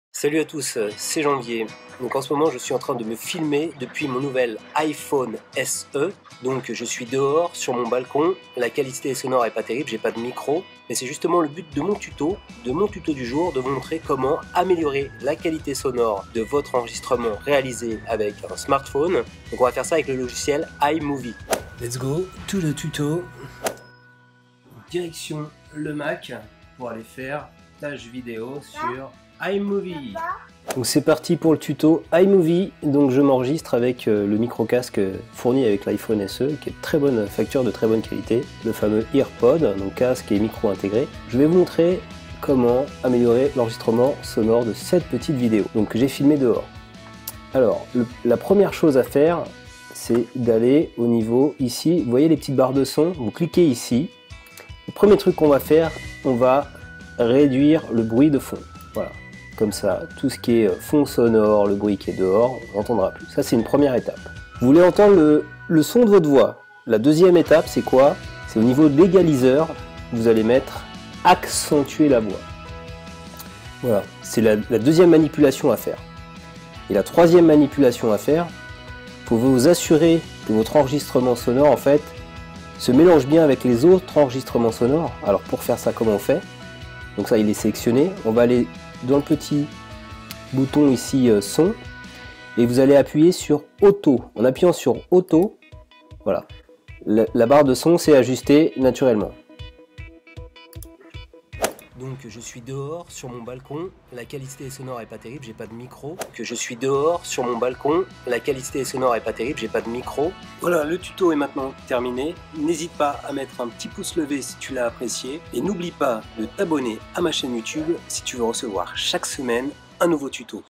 Améliorer le son d'une video iPhone
Tu as enregistré avec ton iPhone une vidéo avec un son de mauvaise qualité ? Gros bruit de fond, voix qui ne porte pas assez, etc... Tout n'est pas perdu ! Avec ce tuto et le logiciel iMovie, tu vas pouvoir rectifier le tir et supprimer le bruit de fond.